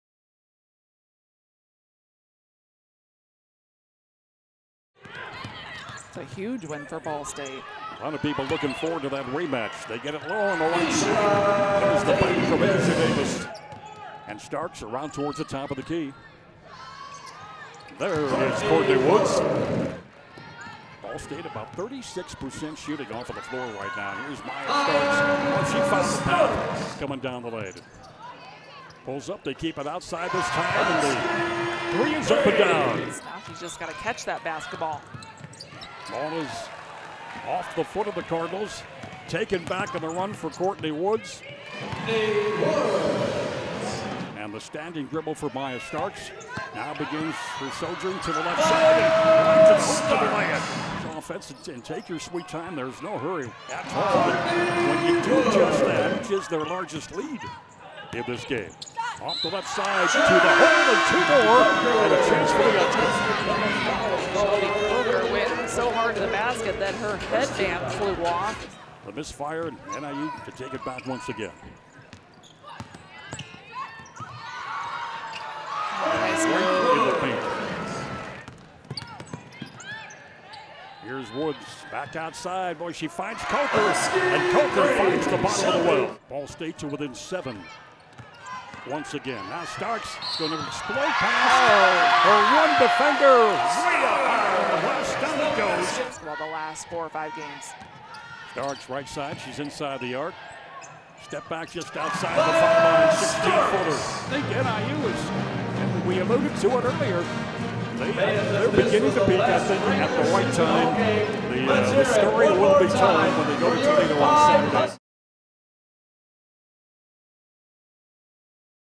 NIU Women's Basketball Highlights - vs. Ball State (3/4/20)
Courtney Woods' fourth double-double of the season (20 points, 11 rebounds) helped lead NIU to a 70-62 win over Ball State at the NIU Convocation Center.